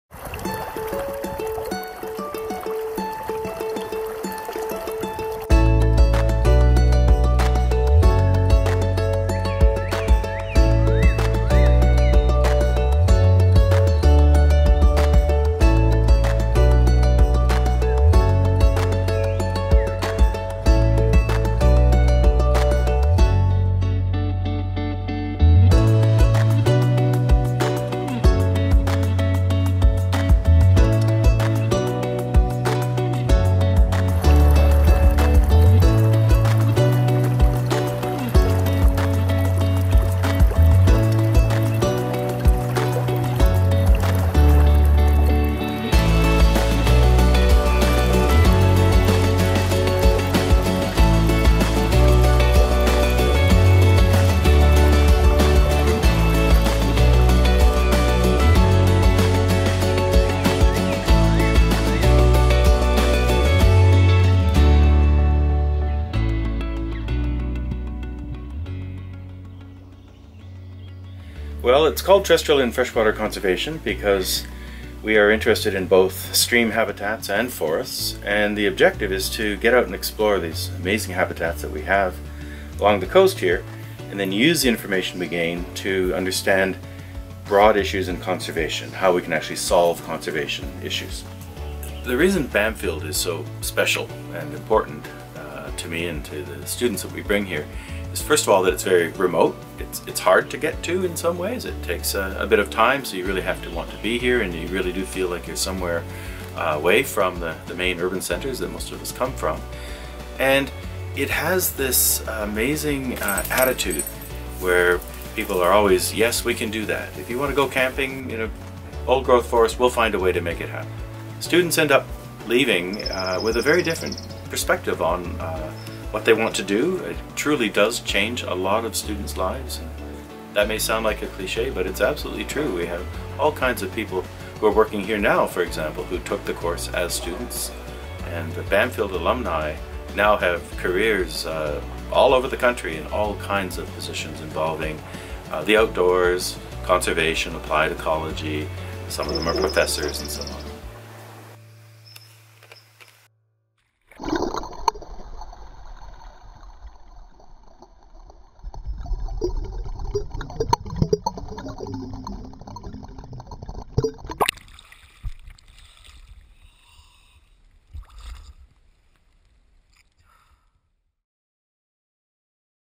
Terrestrial & Freshwater Conservation - BMSC Summer Field Course
This 3-week field course examines modern theory and practice in terrestrial and freshwater habitat conservation ecology. Field experience is supplemented by lectures on global biodiversity, ecosystem services, links between conservation and livelihoods, legislation protecting habitats and species, and local and global solutions.

Students conduct independent research / field projects that include biodiversity surveys, environmental impact assessments and restoration techniques.

2019 Instructor: Dr. John Reynolds (Simon Fraser University)